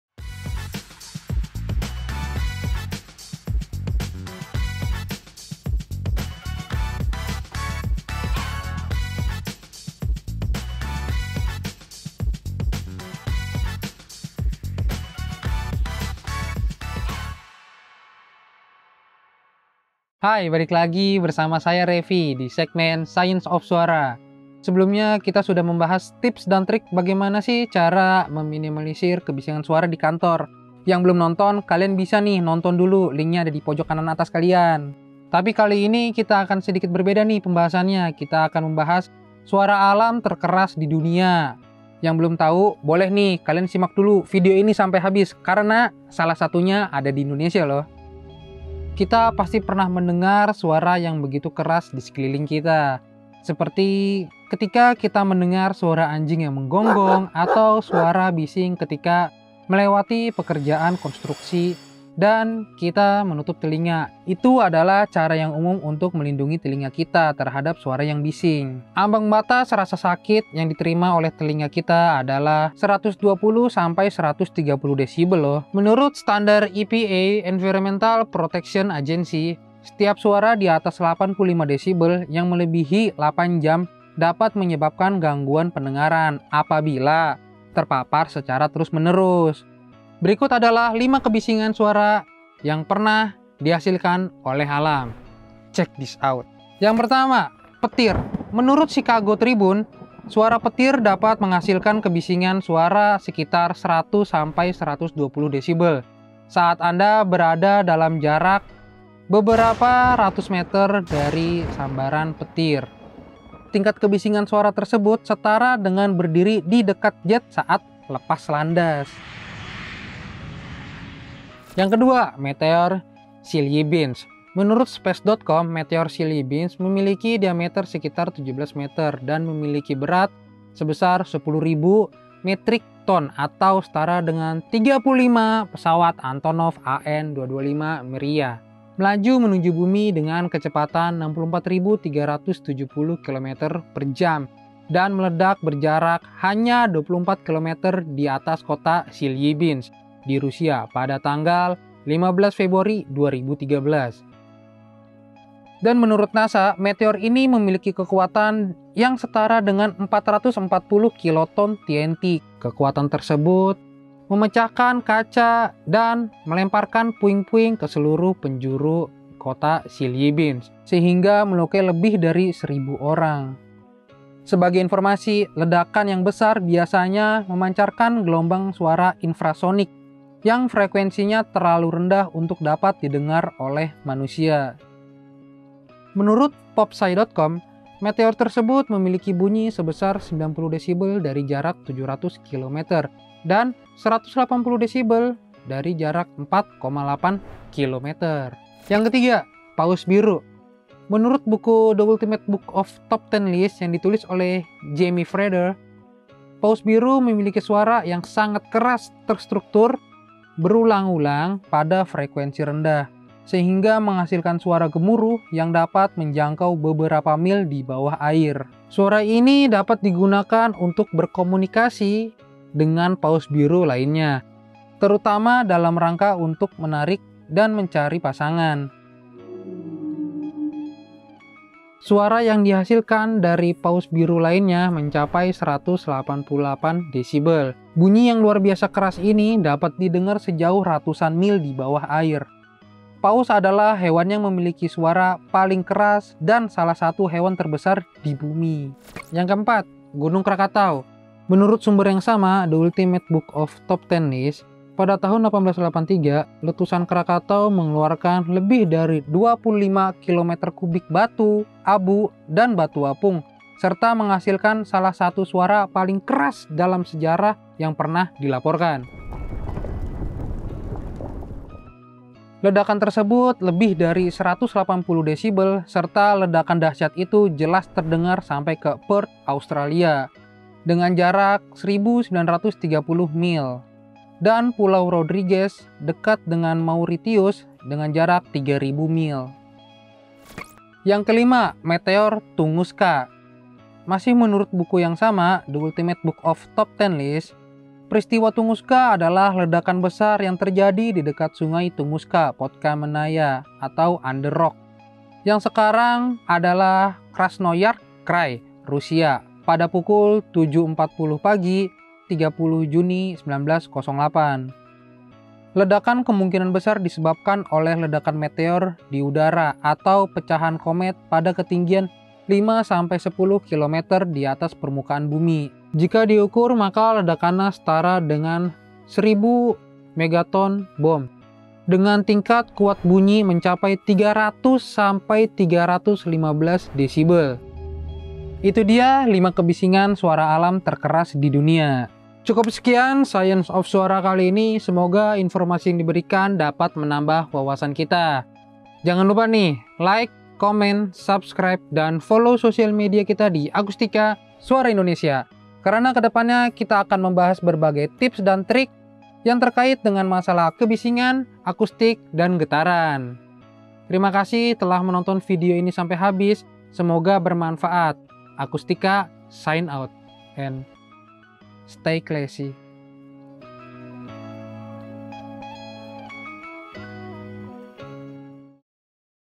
Science of Swara - 5 Suara Alam Terkeras di Dunia
Sebelumnya kita sudah membahas tips dan trik bagaimana cara meminimalisir kebisingan suara di kantor.

Tapi kali ini kita sedikit berbeda nih, kita akan membahas suara alam terkeras di dunia. Yang belum tau boleh nih simak video nya salah satunya dari Indonesia loh.

Kita pasti pernah mendengar suara yang begitu keras di sekeliling kita. Seperti ketika kita mendengar suara anjing menggonggong atau suara bising ketika melewati pekerjaan konstruksi dan Kita menutup telinga, itu adalah cara yang umum untuk melindungi telinga Kita terhadap suara yang bising.

Ambang batas rasa sakit pada telinga manusia adalah 120-130 dB lohh. Menurut standar EPA (Environmental Protection Agency). Setiap suara di atas 85 dB yang melebihi 8 jam dapat menyebabkan gangguan pendengaran "apabila terpapar secara terus-menerus" .

Berikut adalah 5 kebisingan yang pernah dihasilkan oleh alam